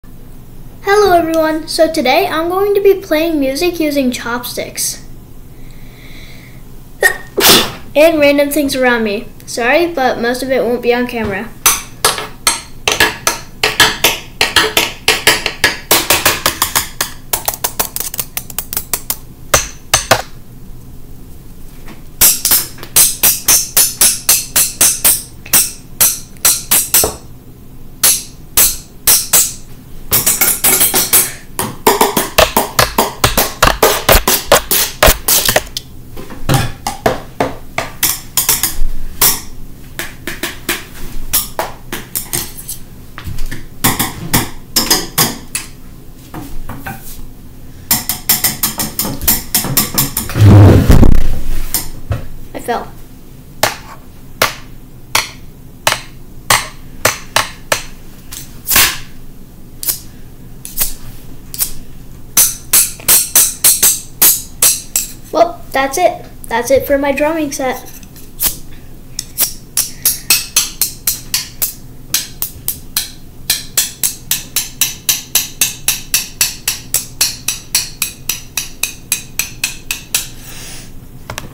I played drums with chop sticks
you read the title but also it was not drums it was random stuff around me